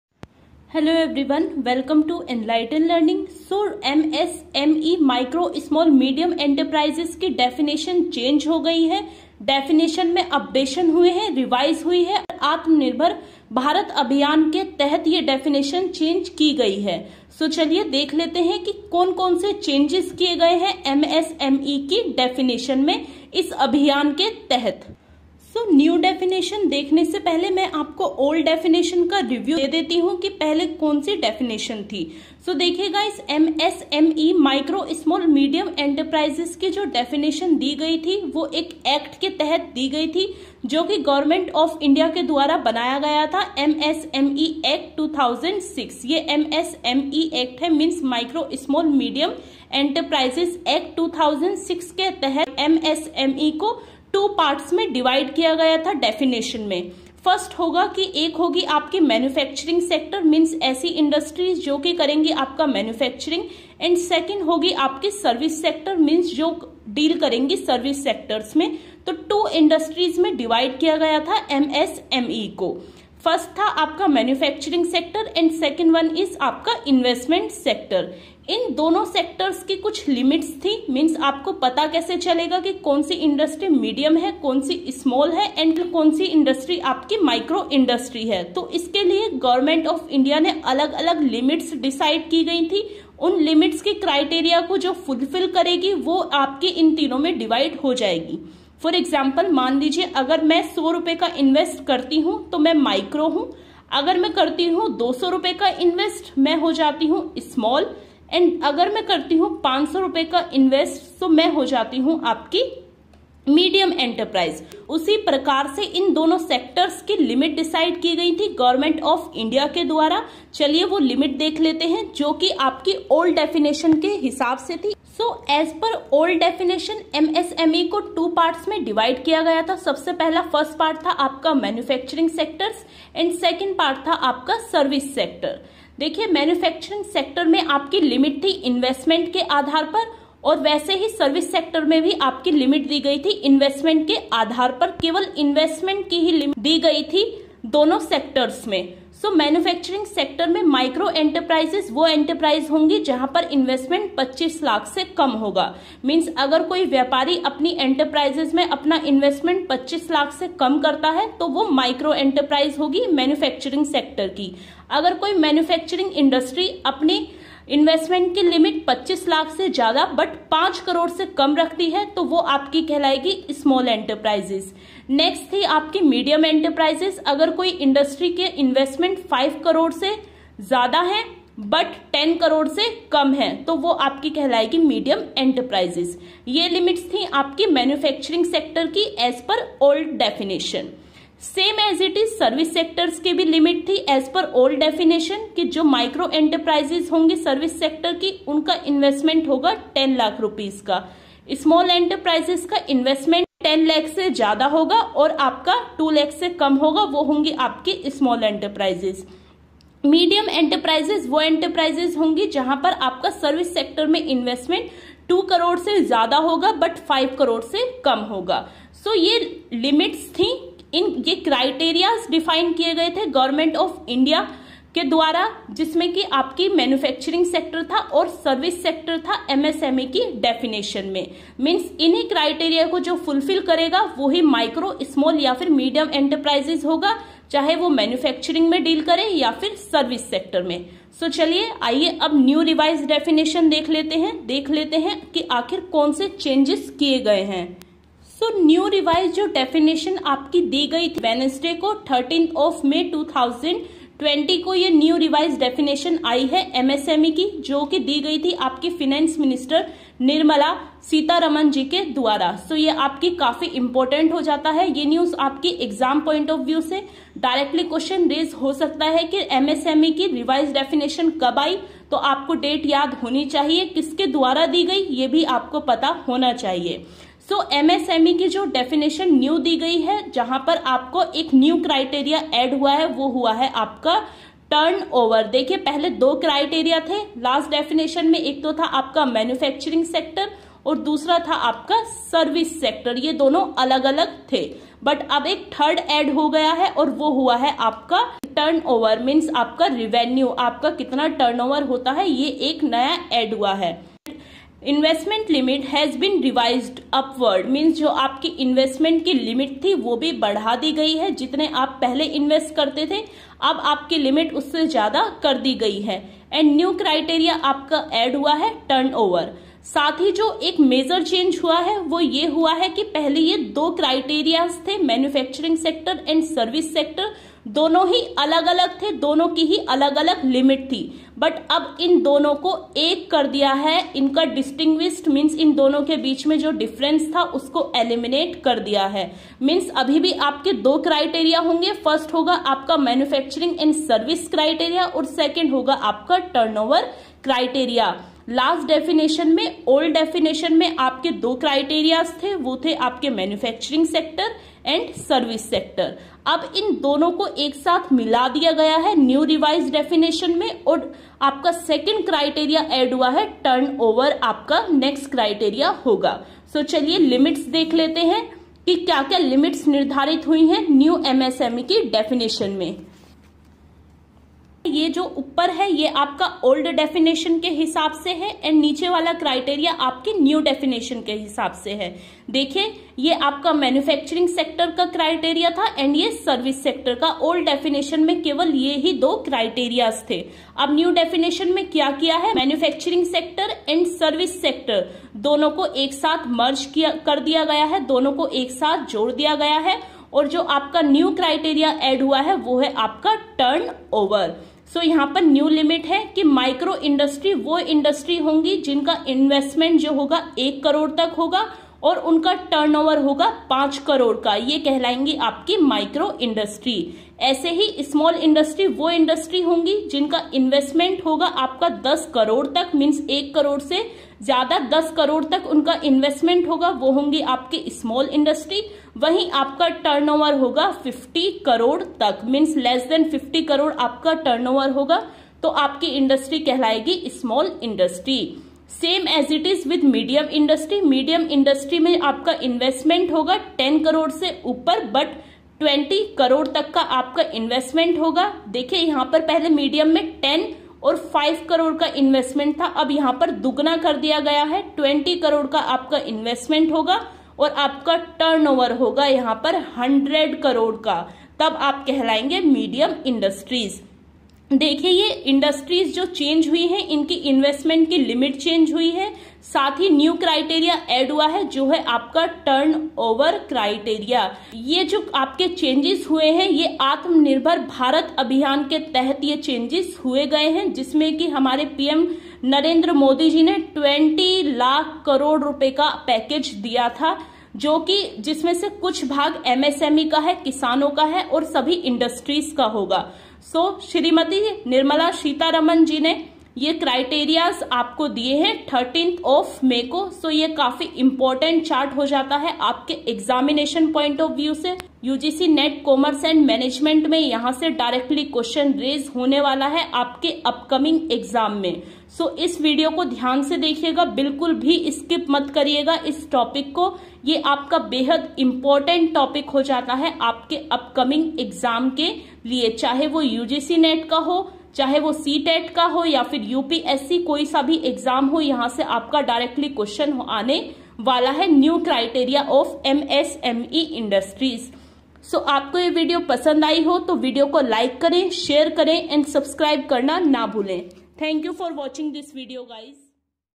MSMEs new Definition, Govt. Changes definition of MSMEs(सूक्ष्म,लघु और मध्यम उद्यम की नई परिभाषा)
In this video I explain about new revised definition of MSMEs which includes new criteria ''Turnover '' 2020 (सूक्ष्म, लघु और मध्यम उद्यम की नई परिभाषा)
This new definition is very much important for your upcoming UGCNET,  SET, UPSC,  and all competitive exam's As Well as For B.COM, M.COM, M.B.A, B.B.A.

enlighten_learning95

If you like this video then Please do